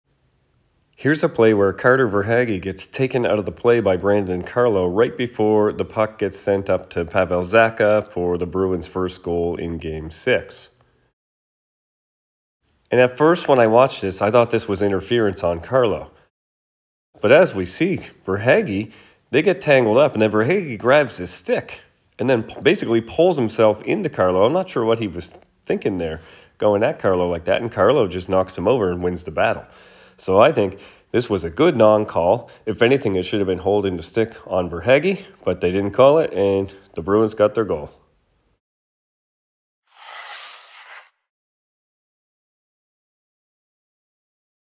Carlo drops Verhaeghe before Zacha goal - Tough Call Review
Brandon Carlo and Carter Verhaeghe got into it just before the puck was sent up the ice for Pavel Zacha to open the scoring in game 6.